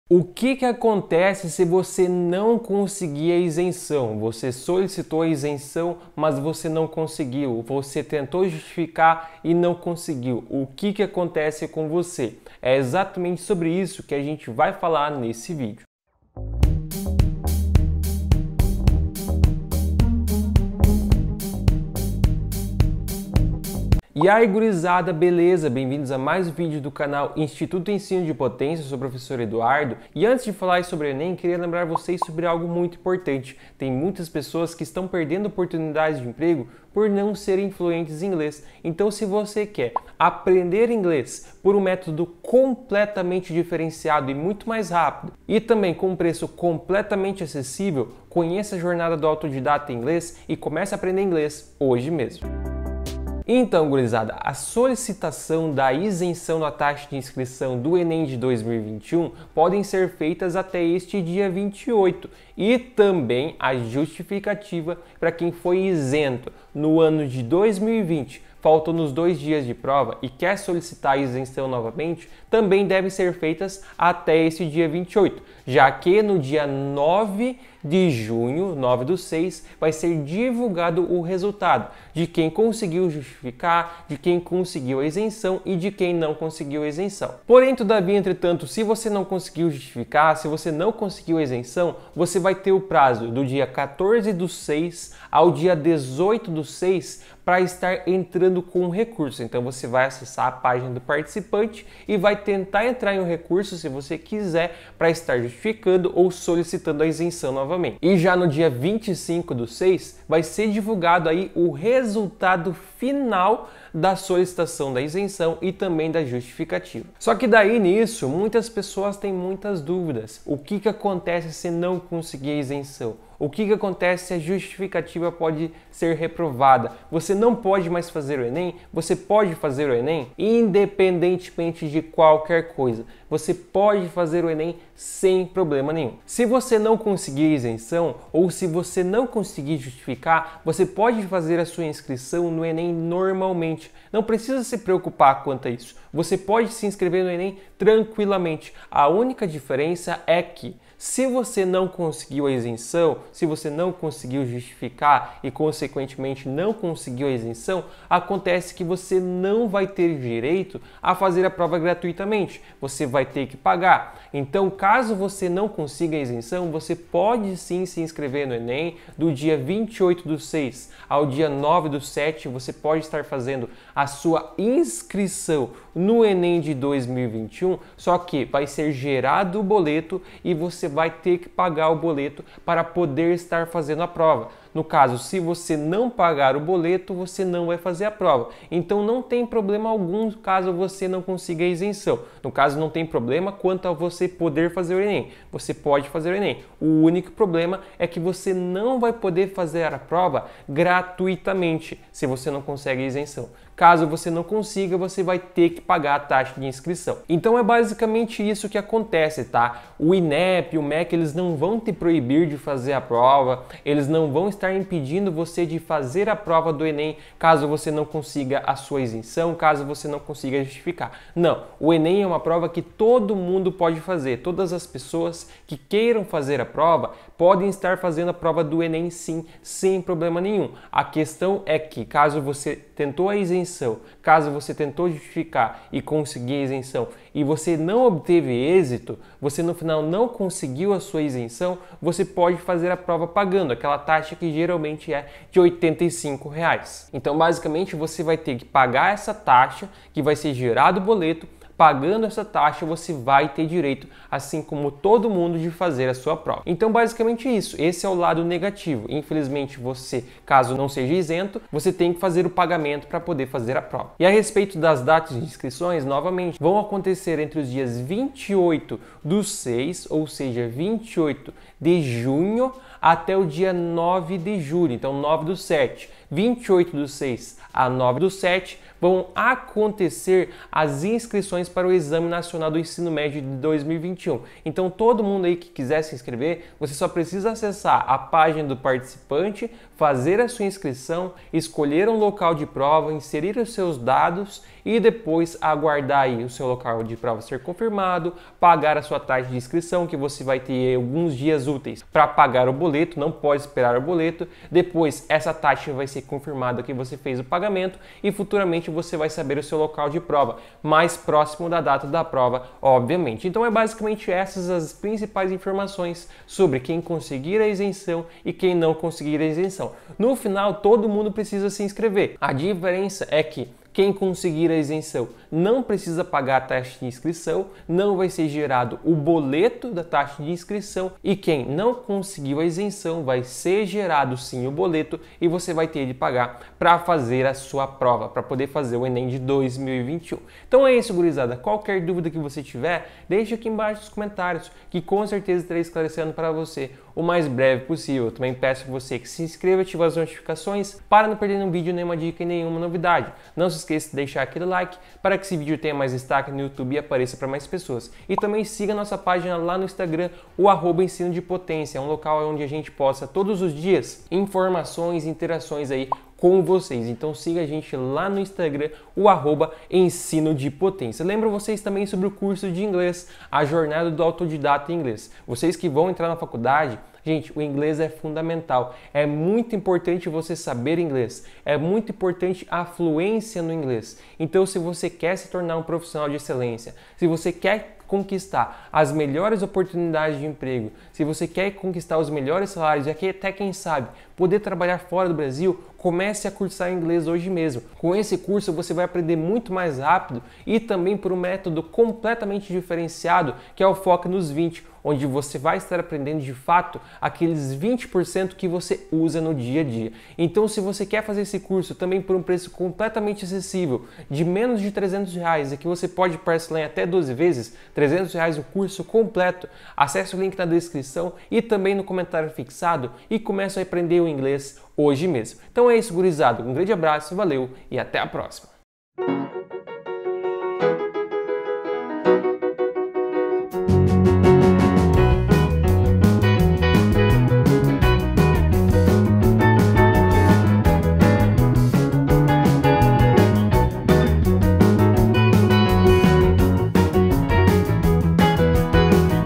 🔥E SE VOCÊ NÃO FOR ISENTO? PODE FAZER O ENEM? | QUAIS SÃO OS PASSOS?
📐ENEM 2021: o que acontece se você não conseguir a isenção? Você não pode fazer o enem se não conseguir o enem 2021? Quanto é a taxa de inscrição?